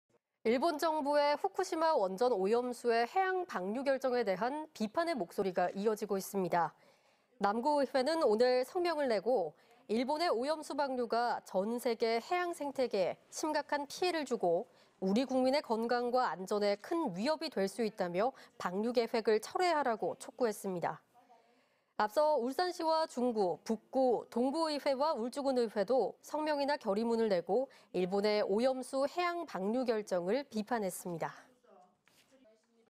남구의회 후쿠시마 원전 오염수 방류 결정 규탄 20210420
남구의회 "후쿠시마 원전 오염수 방류 결정 규탄"

일본 정부의 후쿠시마
원전 오염수의 해양 방류
결정에 대한 비판의 목소리가
이어지고 있습니다.

 남구의회는 오늘(20) 성명을
내고 "일본의 오염수 방류가
전 세계 해양생태계에 심각한
피해를 주고 우리 국민의 건강과
안전에 큰 위협이 될 수 있다"며 방류 계획을 철회하라"고

 앞서 울산시와 중구, 북구,
동구의회와 울주군의회도
성명이나 결의문을 내고
일본의 오염수 해양 방류 결정을 비판했습니다.